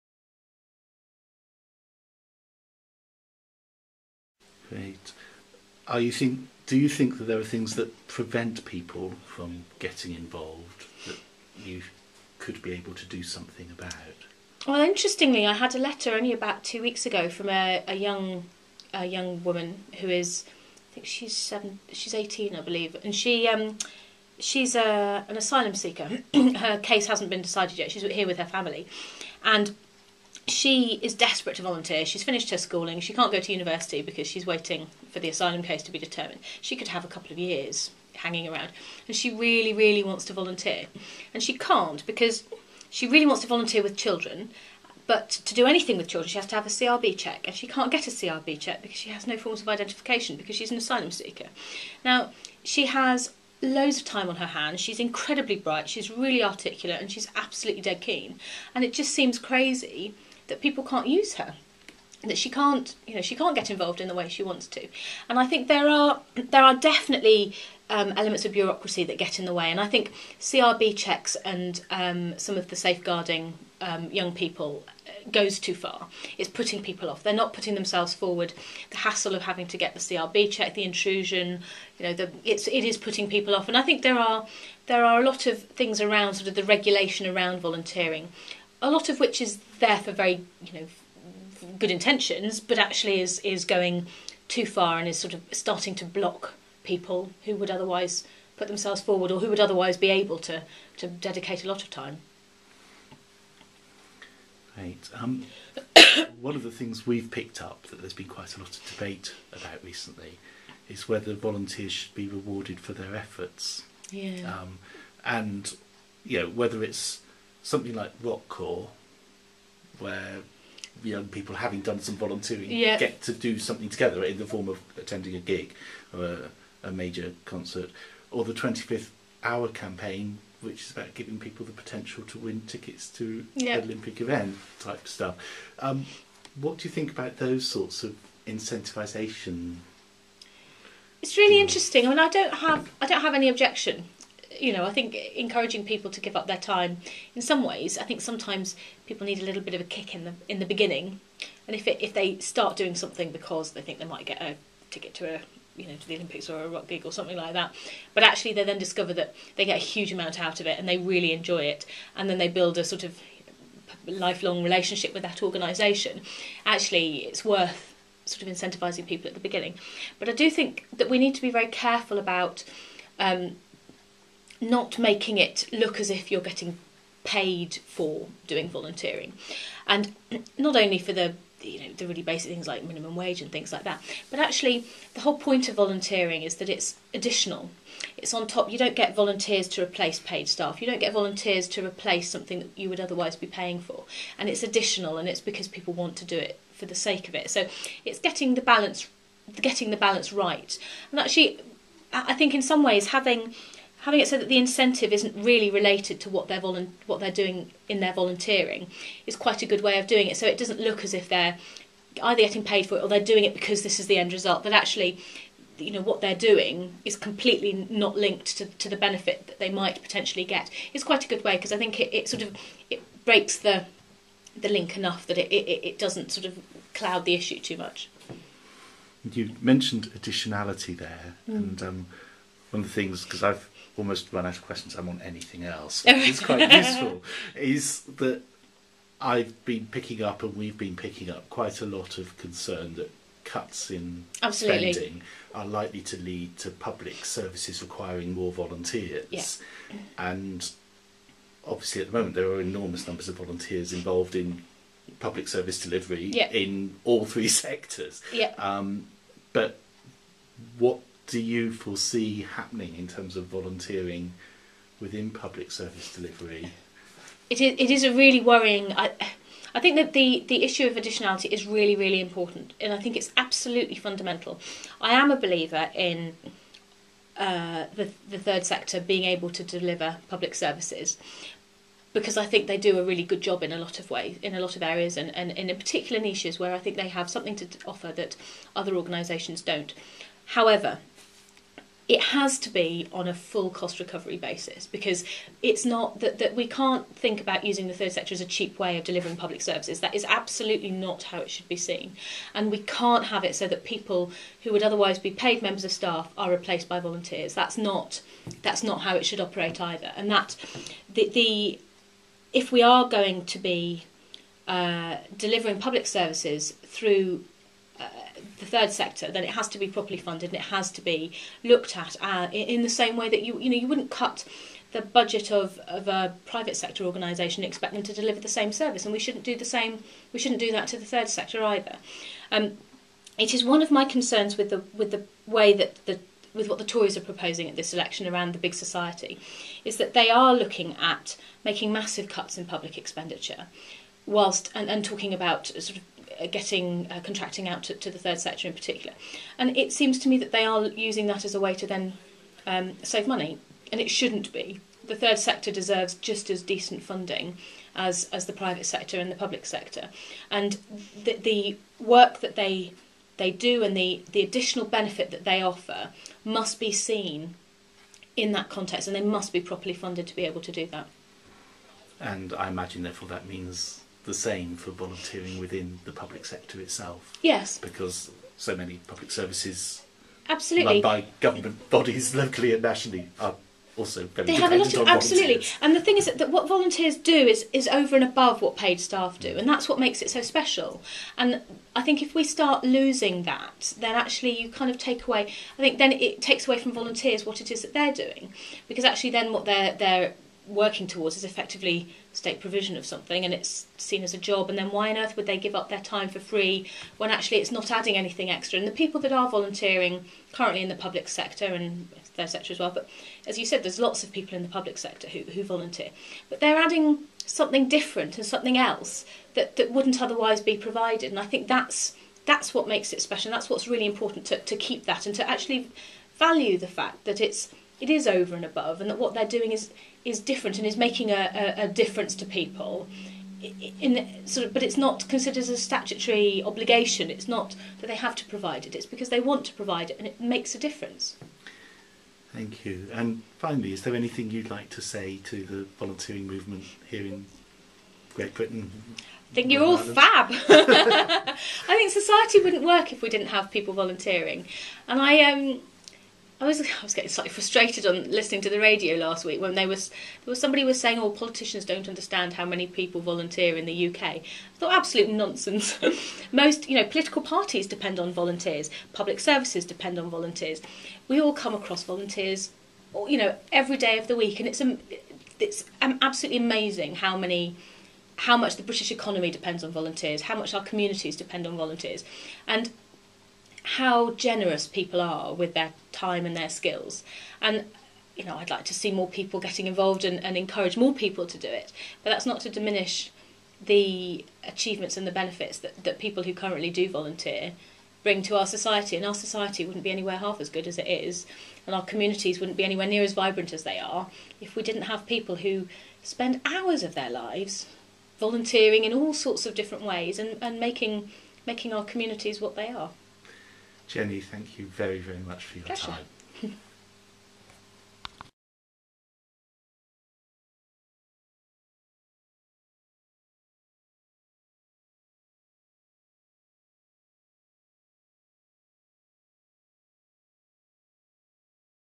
Jenny Willott - 2.wmv
Second part of interview with Jenny Willmott, Liberal Democrat spokesperson on Charities and Volunteering, during the 2010 general election campaign.

The interview is part of a series undertaken for www'i-volunteer.org.uk